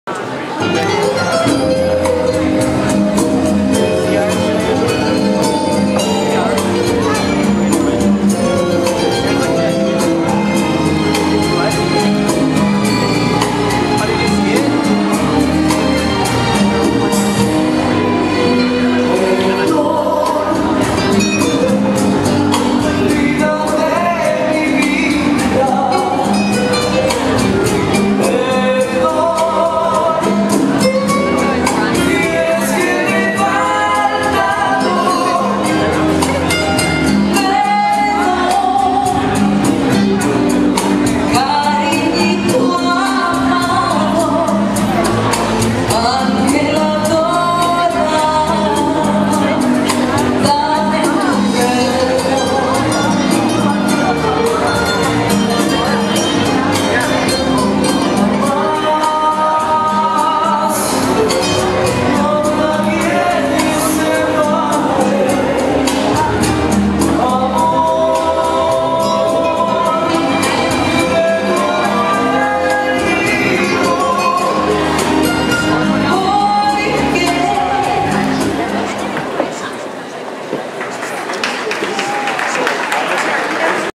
UConn 2008 Newcomer International Rumba - Round 1, Heat 3
UConn ballroom dance competition 2008.